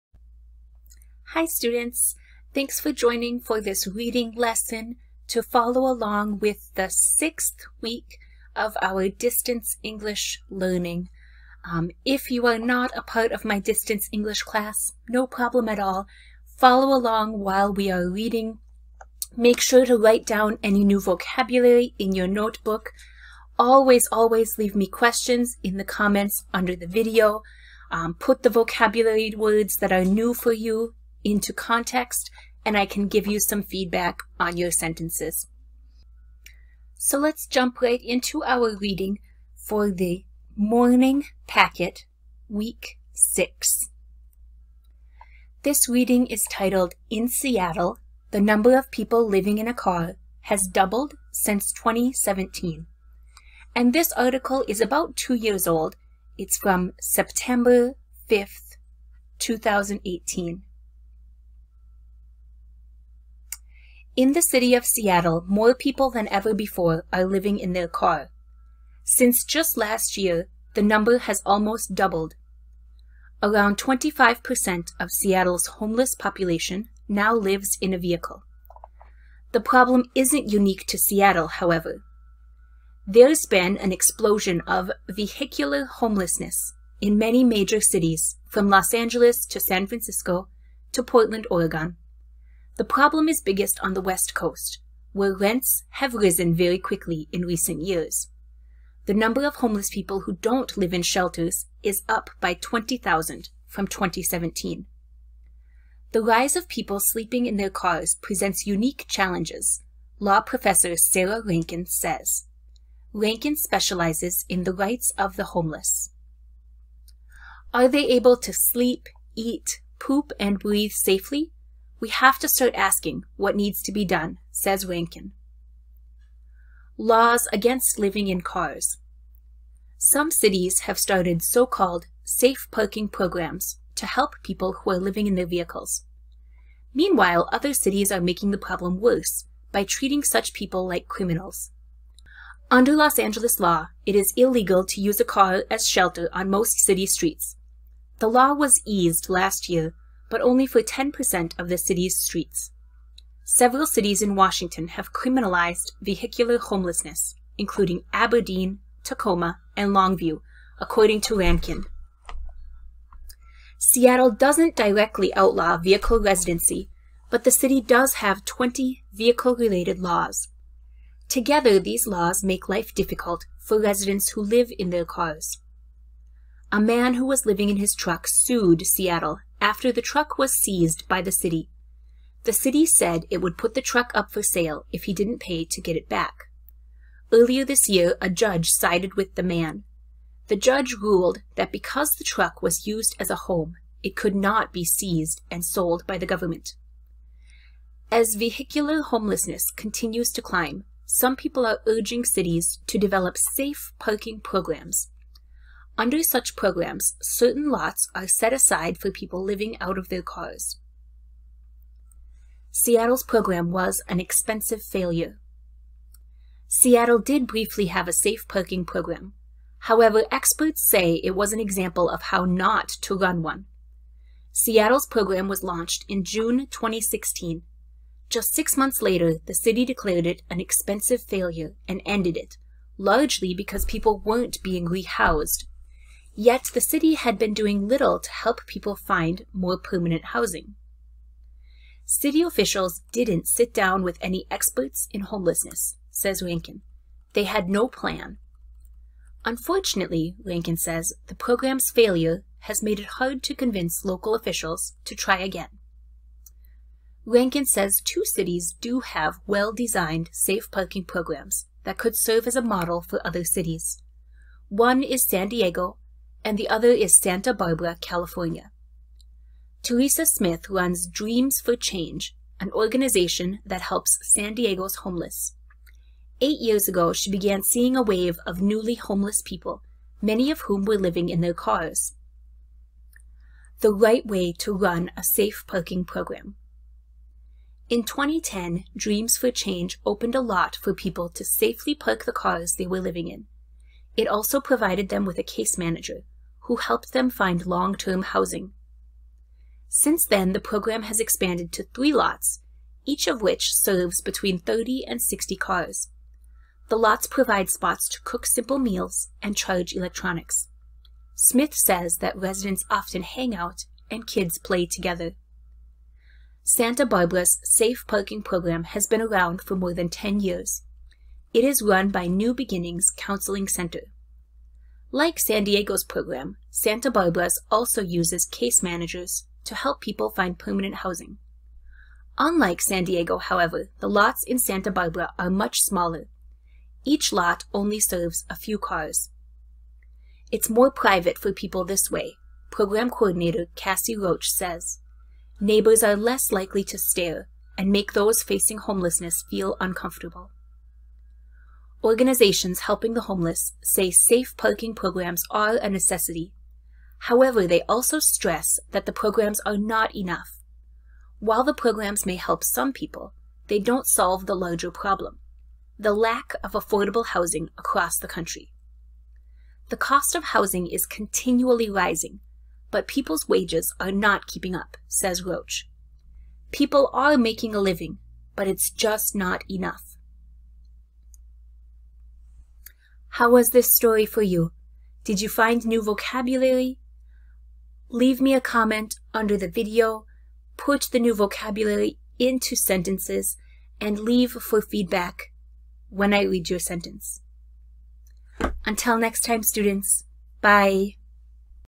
Reading #12 - Living in cars (AM)
This is an audio reading to go along with the reading in week 6 of the morning class distance English packet.  Follow along with your homework and listen to the pronunciation of any new vocabulary.  Leave any questions in the comments below!